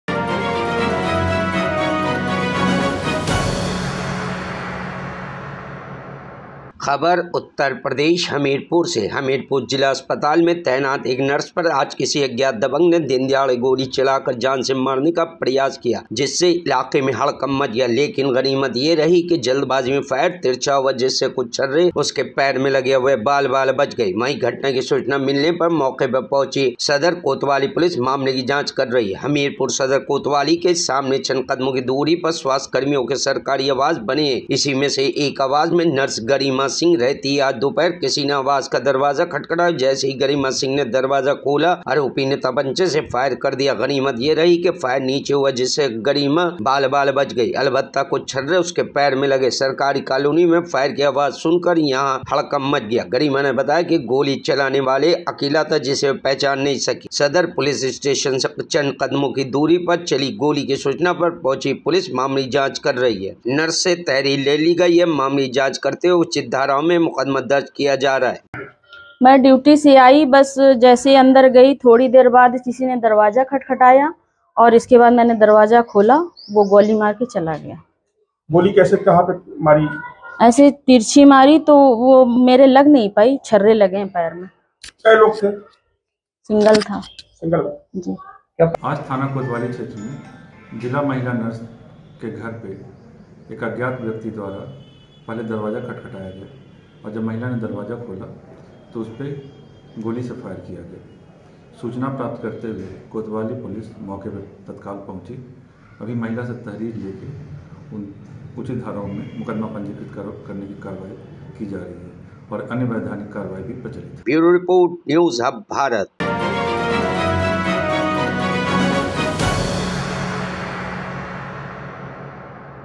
Hamirpur : अस्पताल में तैनात एक नर्स पर आज किसी अज्ञात दबंग ने दिनदहाड़े | NEWS HUB BHARAT
About this video-हमीरपुर सदर कोतवाली के सामने चंद क़दमों की दूरी पर स्वास्थ्य कर्मियों के सरकारी आवास बने हैं। इसी में से एक आवास में नर्स गरिमा सिंह रहती है। आज दोपहर किसी ने आवास का दरवाज़ा खटखटाया और जैसे ही गरिमा सिंह ने दरवाज़ा खोला आरोपी ने तमंचे से फायर कर दिया। गनीमत यह रही फायर नीचे हुआ जिससे गरिमा बाल बाल बच गई। अलबत्ता कुछ छर्रे उसके पैर में लगे हैं। सराकरी कालोनी में फायर की आवाज़ सुन कर यहाँ हडकंप मच गया। गरिमा ने बताया की गोली चलाने वाला अकेला था जिसे वह पहचान नहीं सकी है।